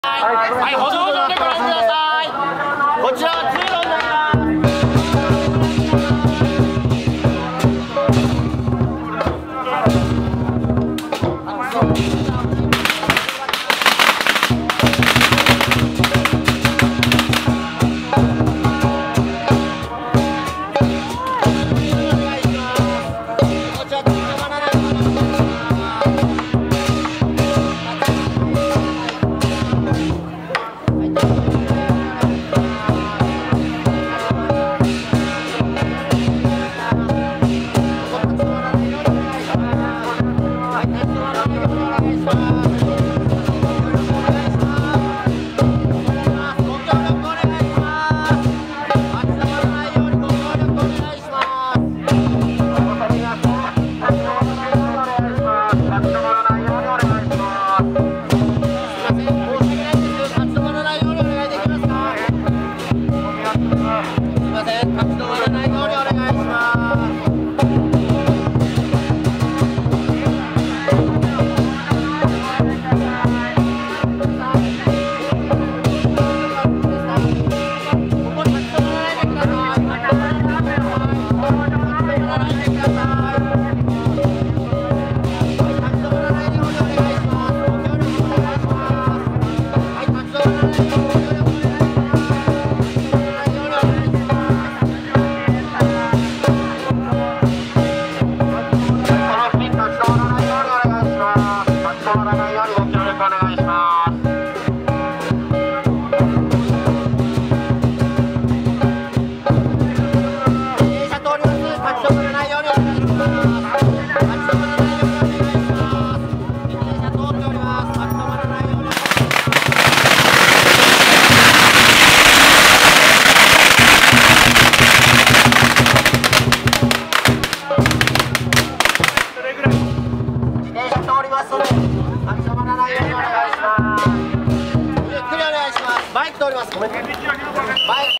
2012年 春節 横浜中華街 採青(さいちん) 2012/01/23 NO.2
新年を祝うと共に富貴吉祥を祈る春節伝統行事です。獅子舞が各店舗の商売繁盛や五穀豊穣を祈願し、店頭に吊るされたご祝儀の紅包(ほんぱお)をくわえとります。